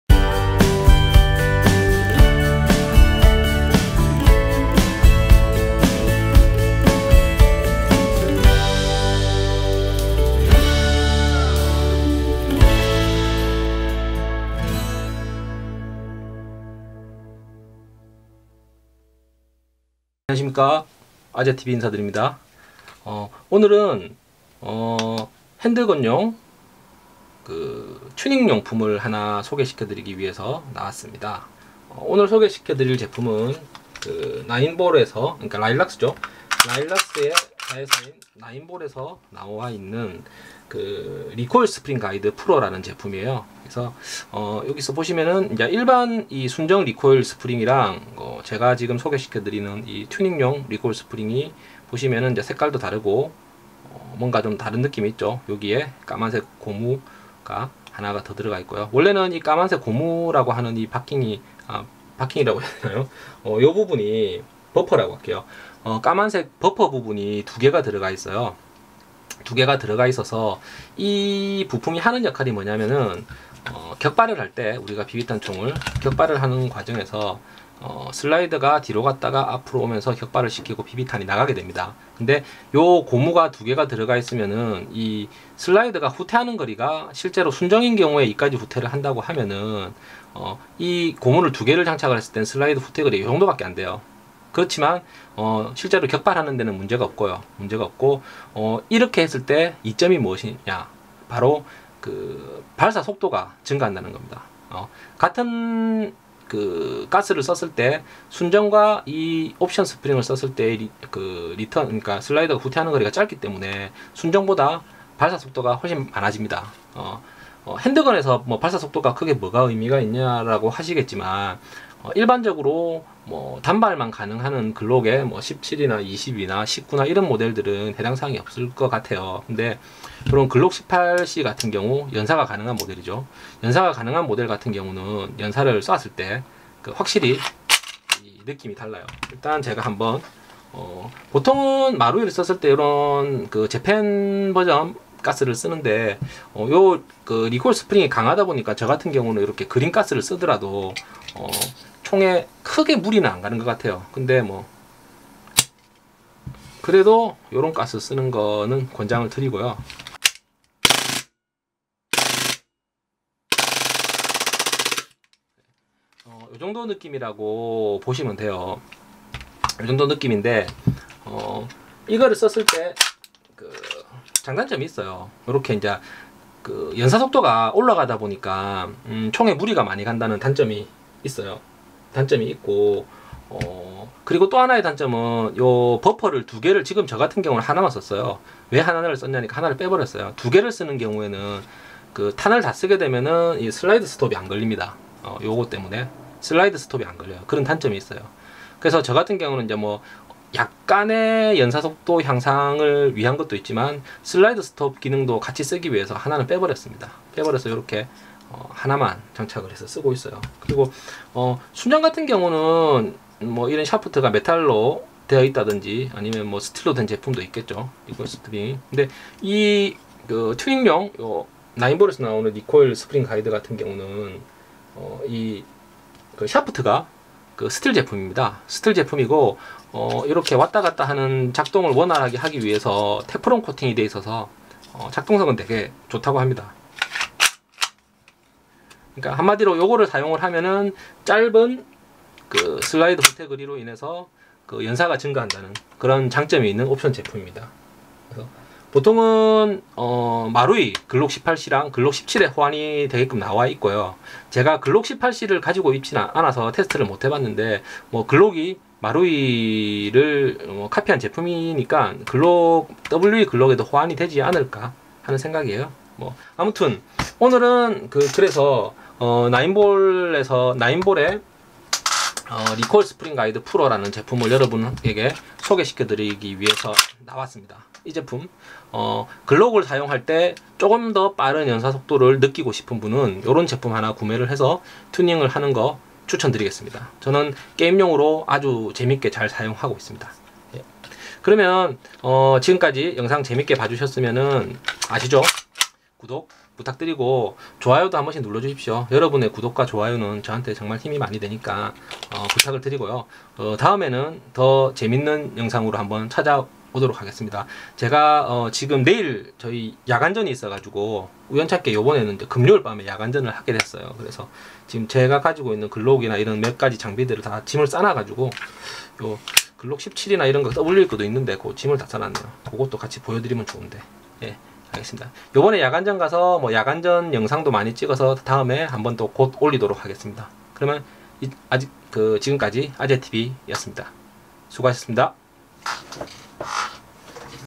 글록 18c 연사속도를 올려보자, Laylax Nine Ball Recoil Spring Guide Pro, 라일락스 나인볼 리코일 스프링 프로
일본 라일락스에서 출시한 나인볼 리코일 스프링 프로
글록모델의 연사속도 또는 단발 속도를 향샹시키는 튜닝용 옵션입니다.